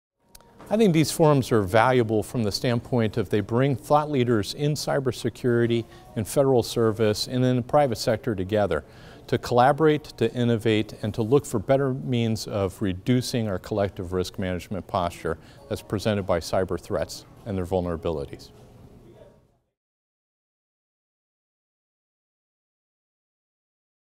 ICIT Interview: Brig Gen (Ret) Greg Touhill, Former CISO, United States
Breg Gen Greg Touhill (Former CISO, United States) discusses the value ICIT provides to public and commercial sector cybersecurity leaders.